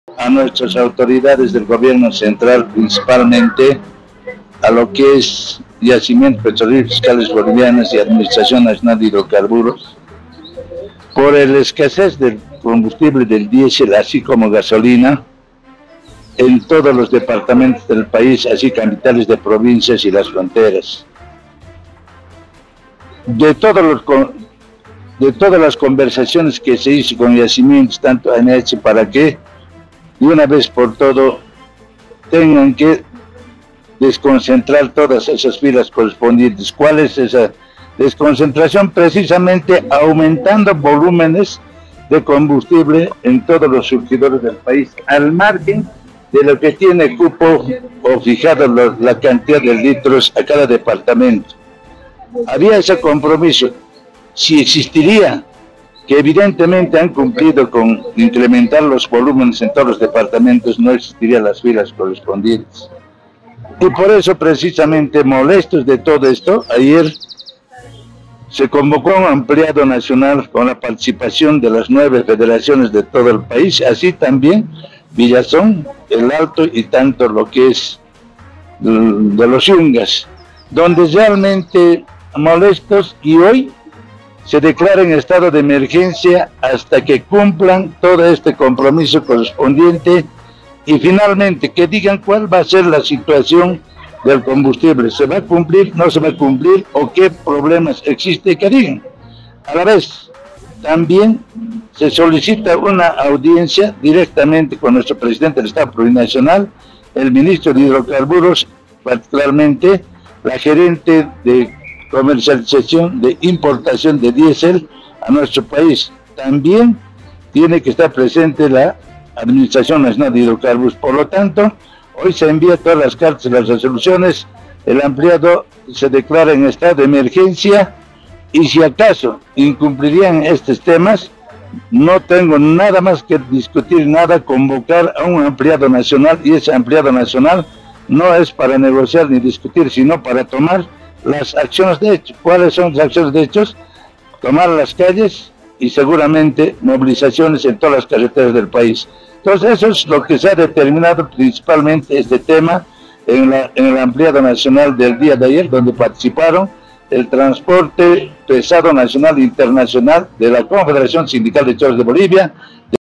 Chóferes de Bolivia se declaran en emergencia por la constante escasez de combustibles en el país
Chóferes de Bolivia se declaran en emergencia por la constante escasez de combustibles en el país y piden reunirse con presidente Luis Arce, informó Lucio Gómez, ejecutivo de la Confederación Sindical de Chóferes de Bolivia.